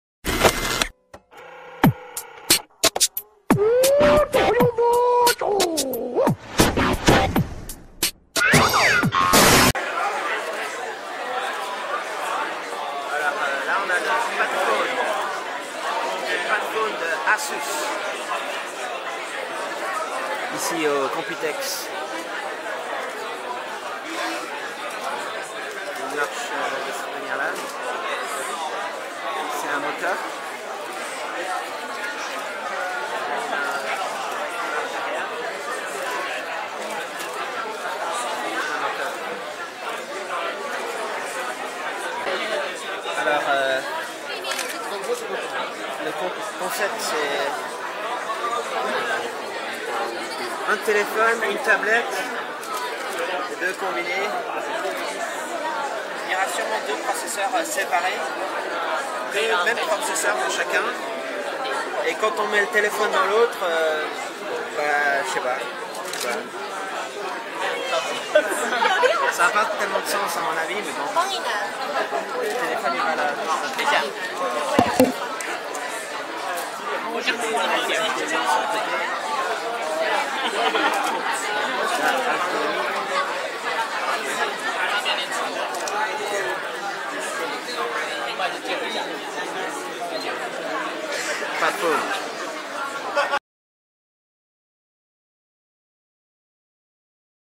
Asus Padfone (FrAndroid)
Petite vidéo de l'Asus Padfone par Charbax (armdevices) pour FrAndroid au Computex